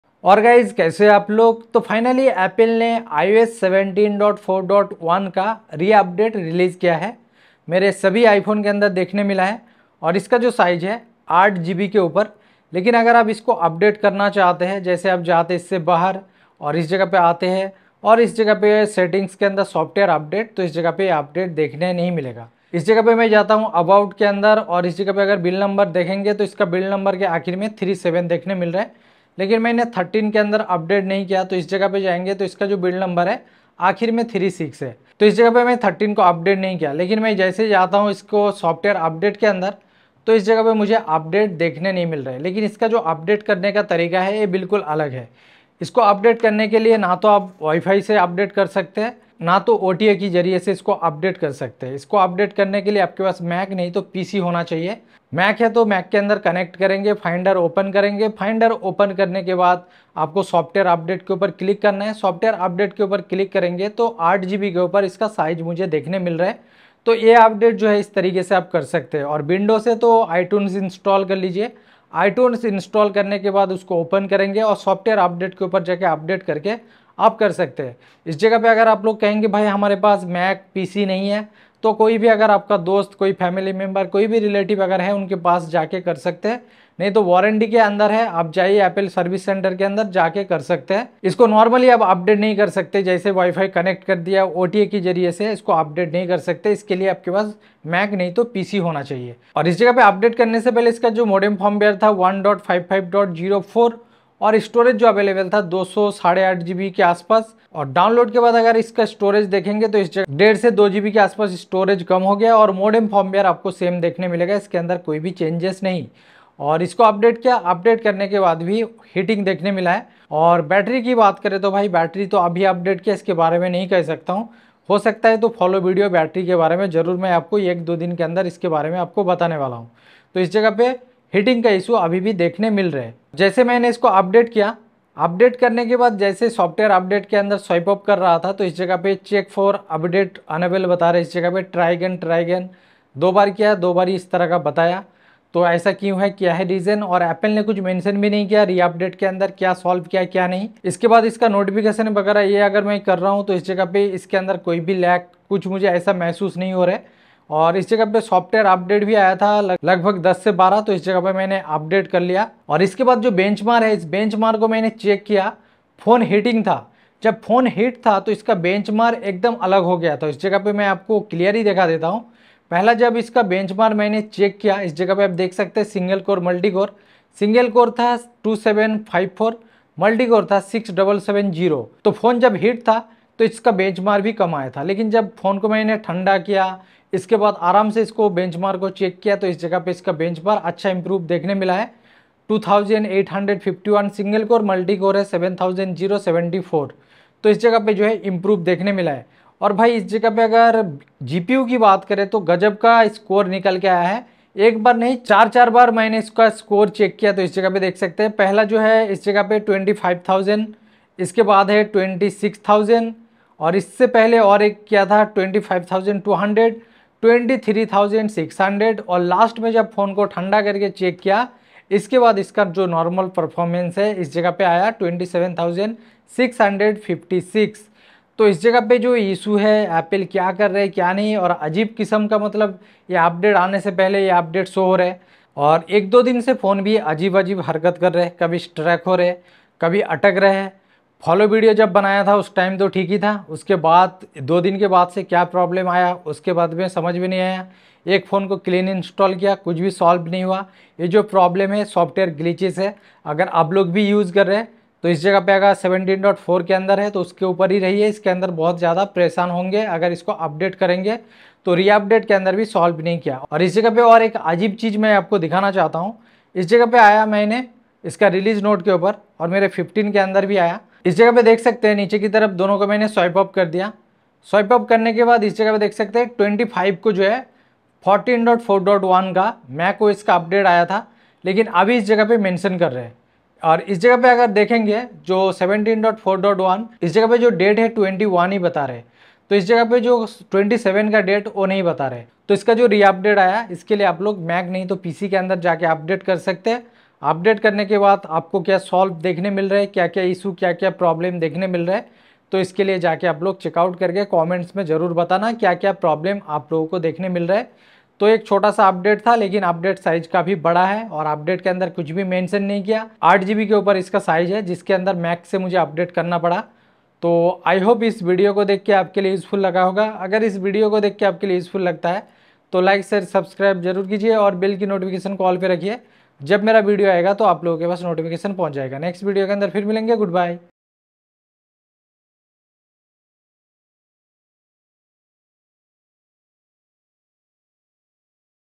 iOS 17.4.1 | iOS 17.4.1 Re Update - Released Big Problems in Hindi
In this video iOS 17.4.1 Re Update Released Bug Fixes in iPhone.

Watch this video till end to know everything about this iOS 17.4.1 RE!